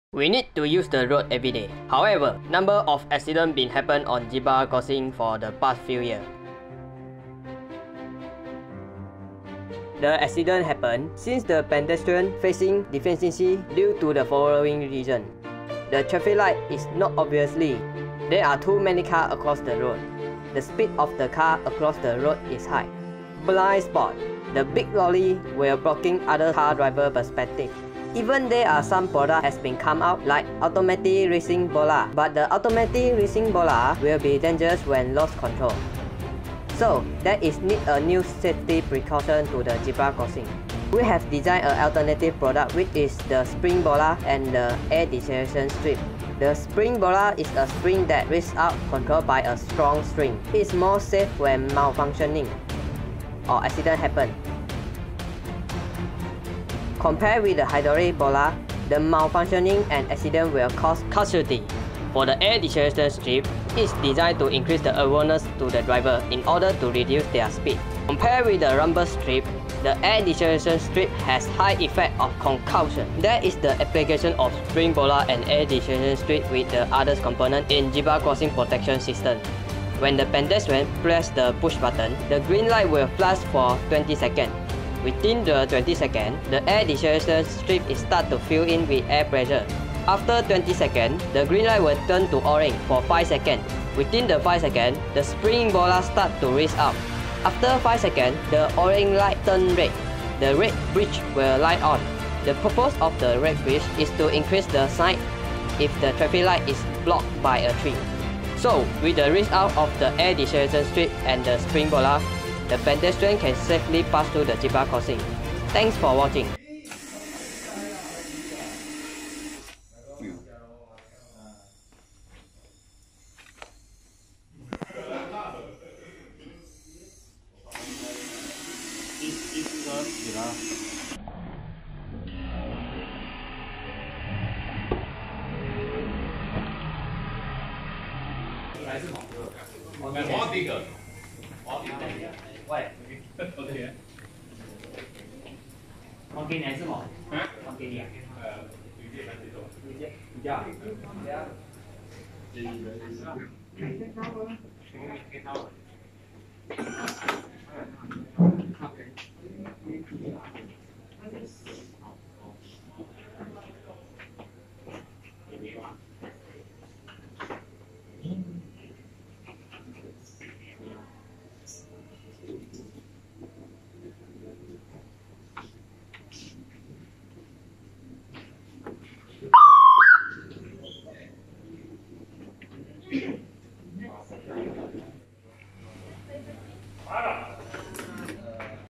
Safety implementation of Zebra Crossing(James dyson)
Number of accident happened on zebra crossing system for the past few years. Recently a 10 years old pupil from Hwang Hwa primary school was knocked down by a car while using the pedestrian crossing. The hit and run case did happen at zebra crossing located in front of Polis Station of Pulau Tikus, Pulau Pinang. The current zebra crossing pedestrian faced deficiency due to the following reasons:
(i) Surface of road
(ii) Blindspot cars
(iii)  Traffic light not obvious
(iv) Too many cars
(v) Speed car very high

Zebra crossing protection system is aimed to protect the pedestrian from un-concentrated car driver.